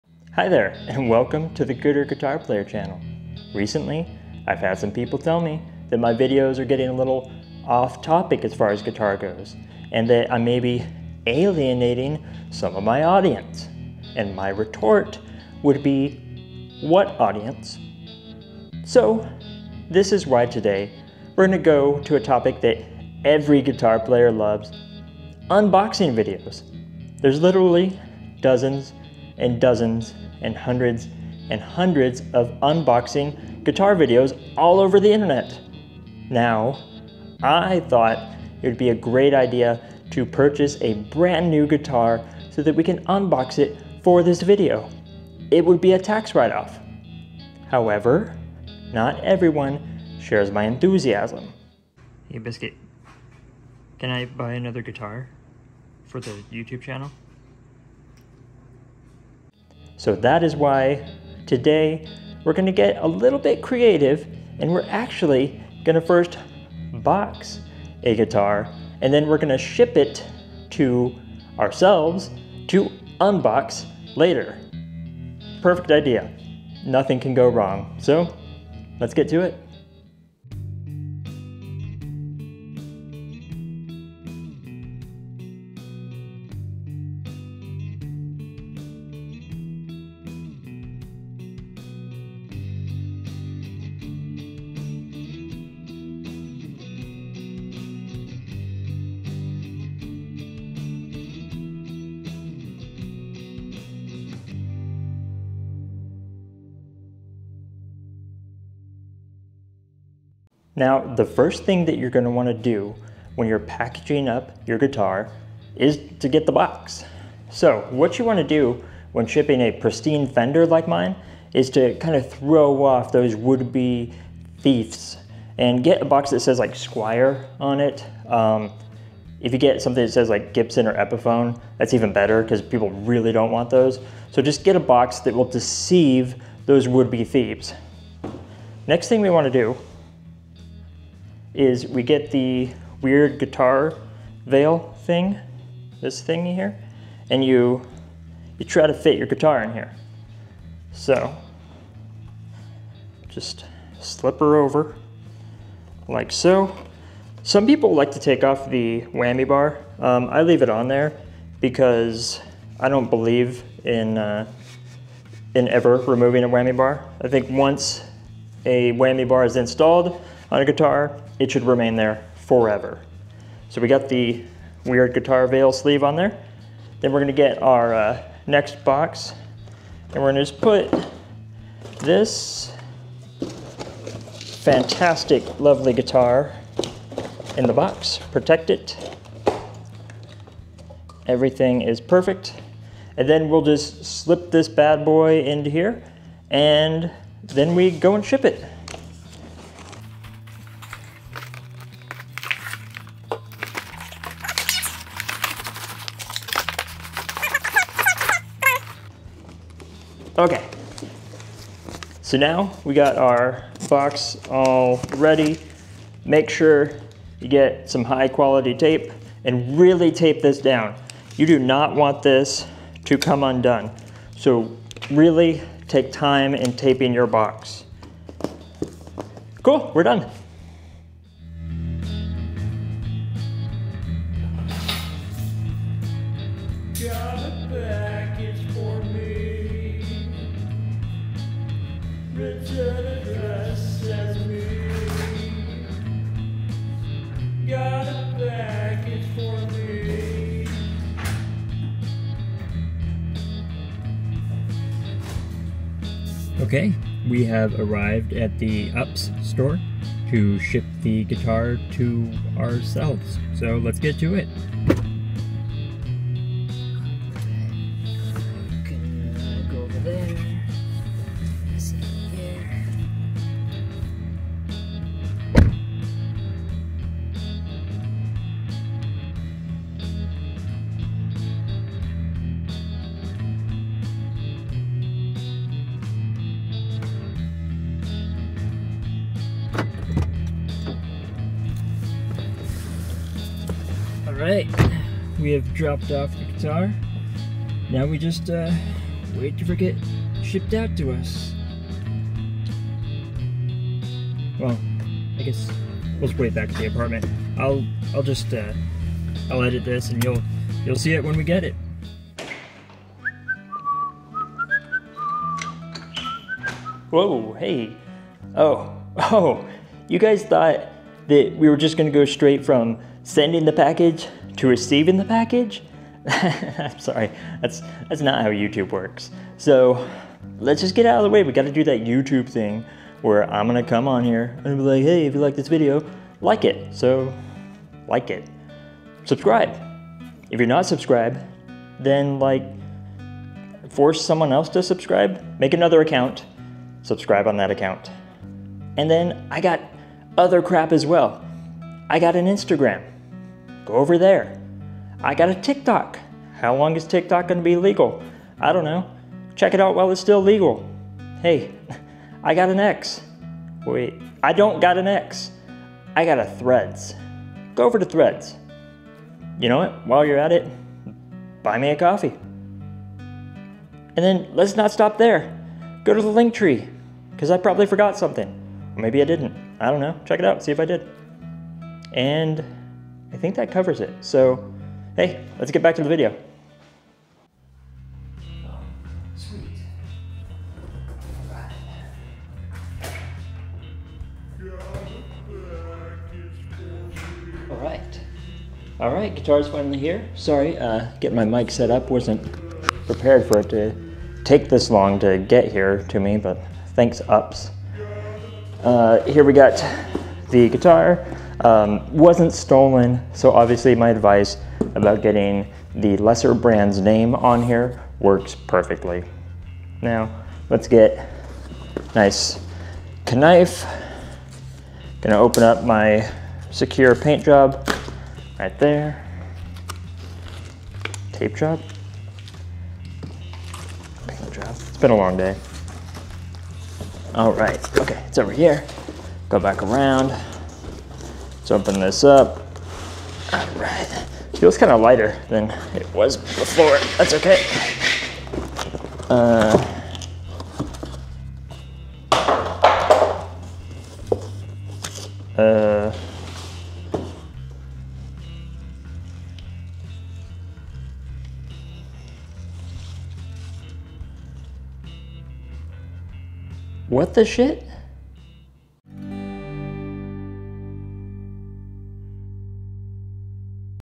A Gooder, guitar unboxing!
In a true return to form Gooder Guitar player is excited to give the fans exactly what they want. AN UNBOXING VIDEO!

0:00 intro before the intro
1:21 Intro thing
1:51 if you want to skip the intro…I mean I worked hard on it but what ever, packing starts here I guess.
3:36 don’t forget the tape
3:52 Shippin’!
5:13 sorry…gotta take care of Bidnis
6:28 Got a package…from me